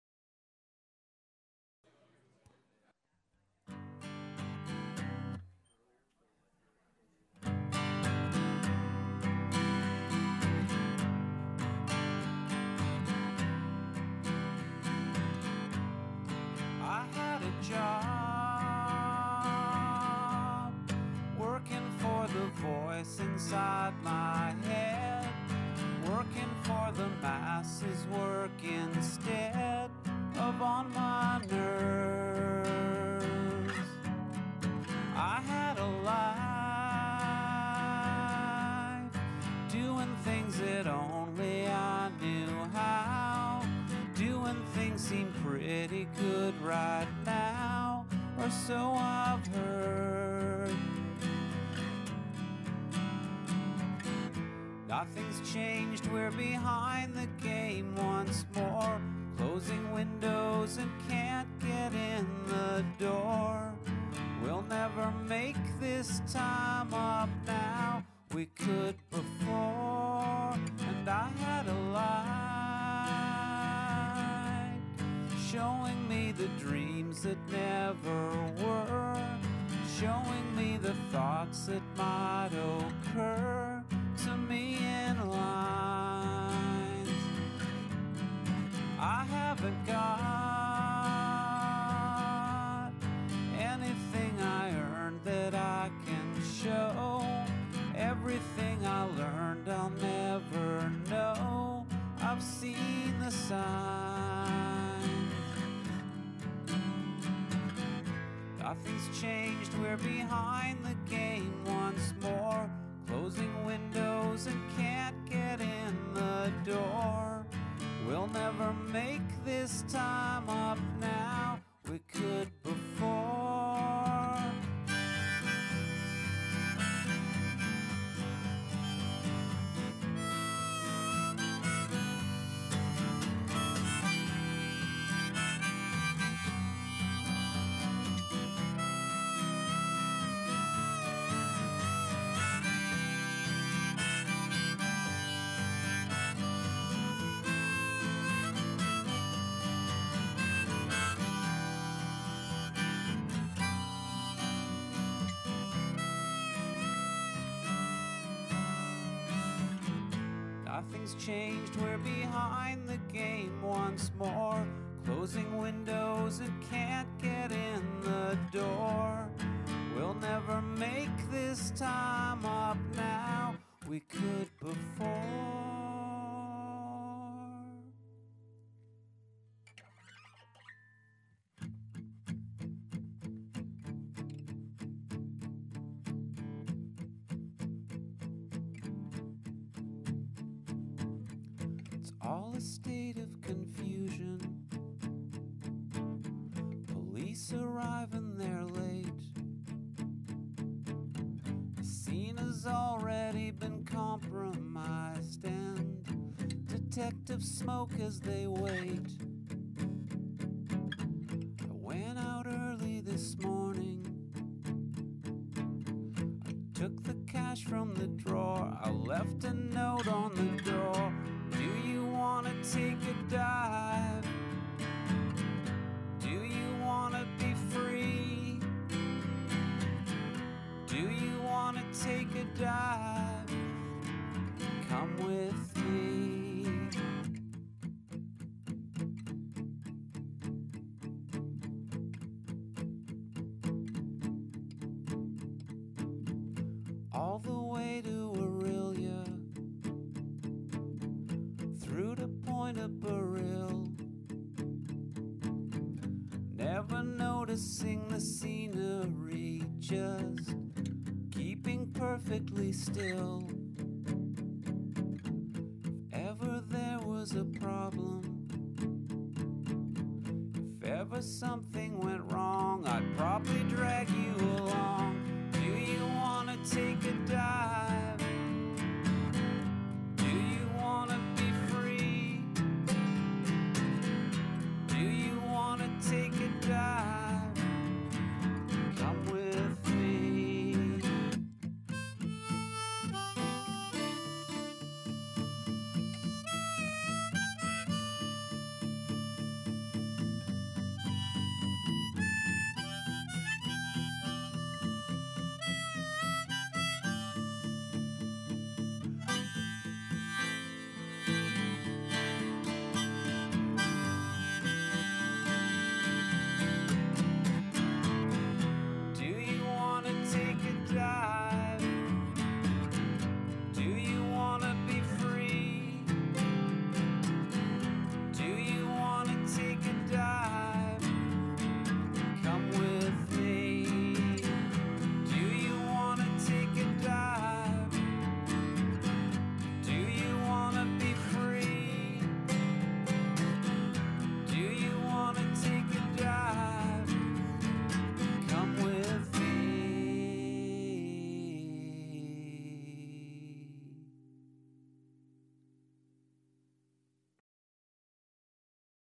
The Brown Bottle Sessions: the Priddle Concern
The Priddle concern playing at The Verge's Brown Bottle Sessions